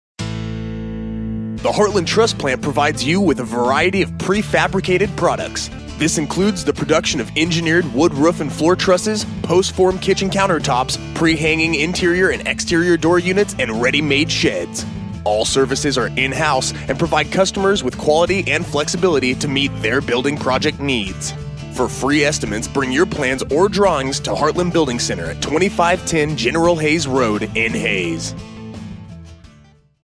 HBC Truss Plant Drone
All trusses on this project were custom made by Heartland Truss Plant in Hays, owned & operated by Heartland Building Center.